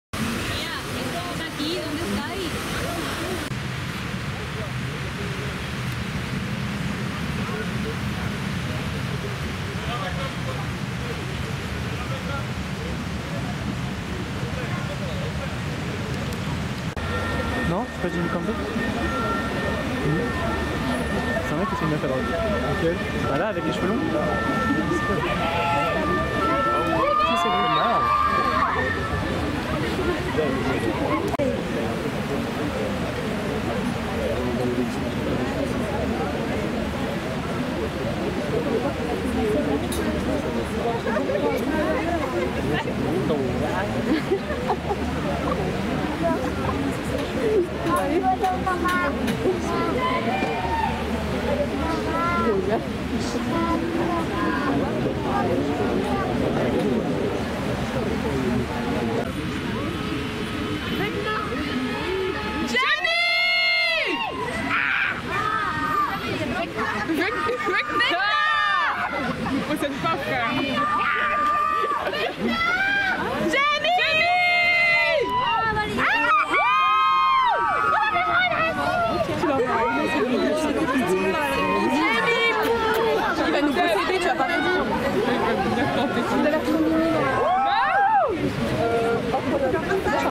Jamie Campbell Bower [Stranger Things / Twilight] - Saint Laurent SS 2023 show in Paris - 27.09.2022
British actor and singer Jamie Campbell Bower attending the Saint Laurent Spring Summer 2023 fashion show in Paris, September 27th 2022.